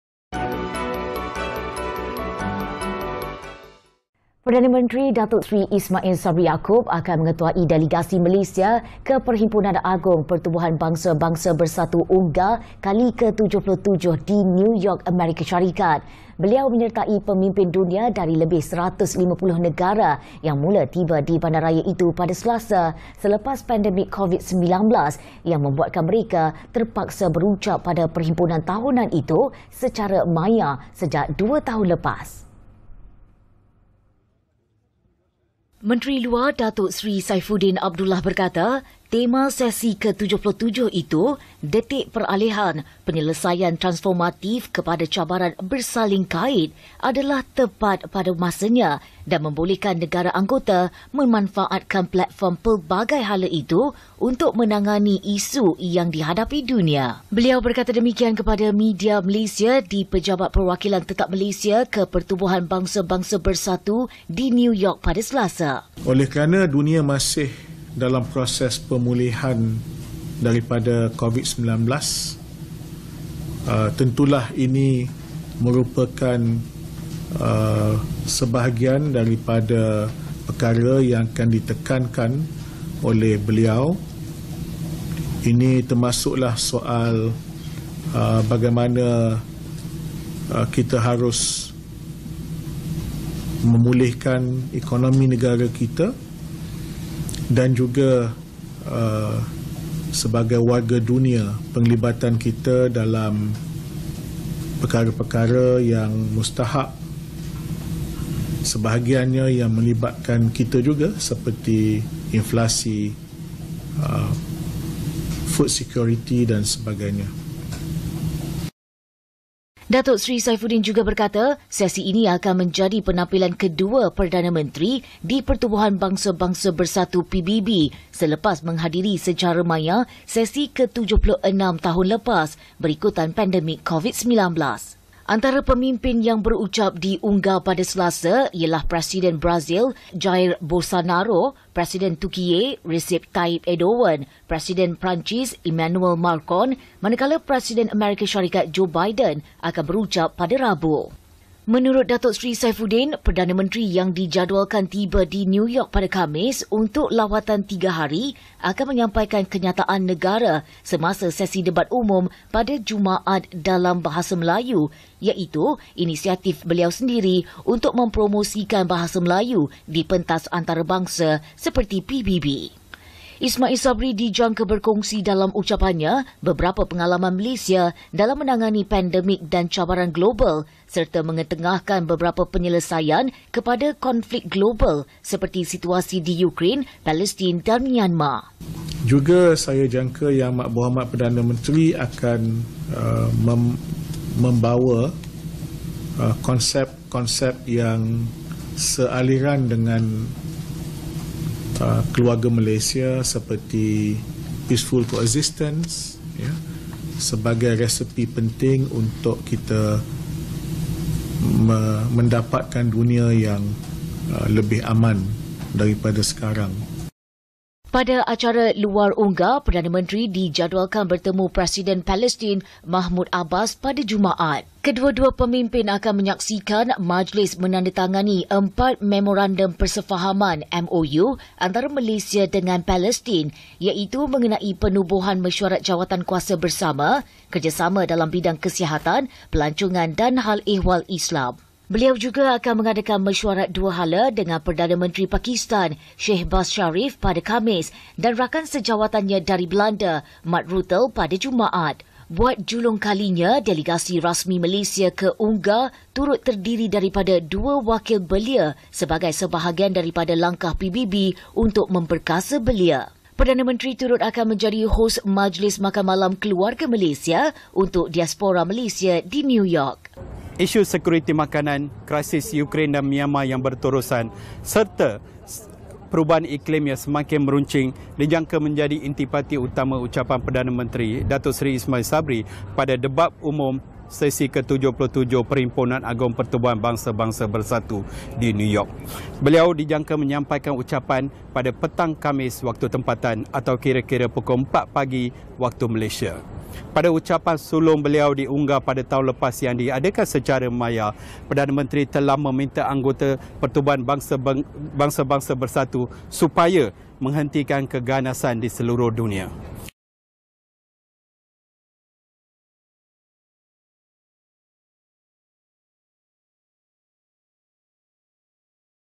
UNGA77 | YAB Perdana Menteri Ketuai Delegasi Malaysia
YAB Perdana Menteri, Dato' Srri Ismail Sabri Yaakob akan mengetuai delegasi Malaysia ke Perhimpunan Agung Pertubuhan Bangsa-Bangsa Bersatu (UNGA) ke-77 di New York, Amerika Syarikat.

Beliau menyertai pemimpin dunia dari lebih 150 negara yang mula tiba di bandaraya itu pada Selasa, selepas pandemik COVID-19 yang membuatkan mereka terpaksa berucap pada perhimpunan tahunan itu secara maya sejak dua tahun lepas.

Sumber : Bernama